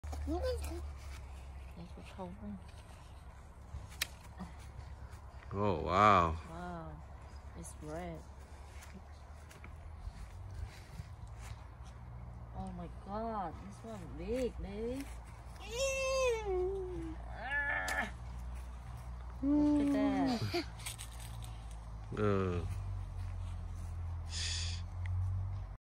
Picking persimmons
Harvesting some persimmons. I have this tree for 5 years. This is the first time that, it stopped dropping fruits